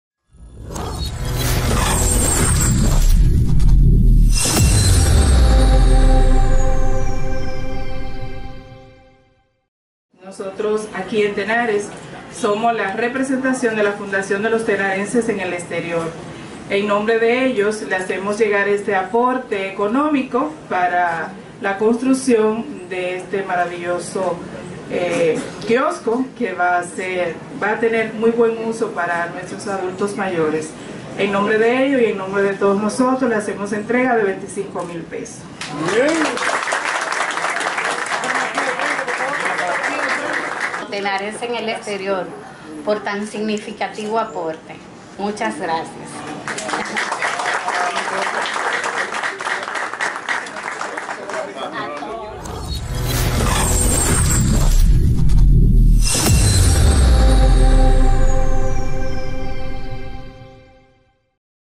Fundación Tenarenses en el Exterior realiza significativo aporte a la Casa Comunitaria Dr Manuel Te
La Fundación Tenarenses en el Exterior,  comprometida con las mejores causas de nuestro municipio, realizó la entrega de 25,000 pesos a la Casa Comunitaria Dr. Manuel Tejada Florentino, esta contribución fue entregada durante el gran evento Dulce Encuentro, en el cual se buscaba recaudar los fondos para la construcción de un Kiosko que le dará calidad de vida a los adultos mayores de esa importante institución social; la entrega del aporte fue realizada por Josefina Polanco presidenta del Comité en Tenares de esta importante Fundación y parte de los miembros de la directiva de Tenares, y fue recibida por Ana González presidenta del consejo directivo de la Casa Comunitaria y por Ana Mercedes Arias presidenta de ASOPRODETE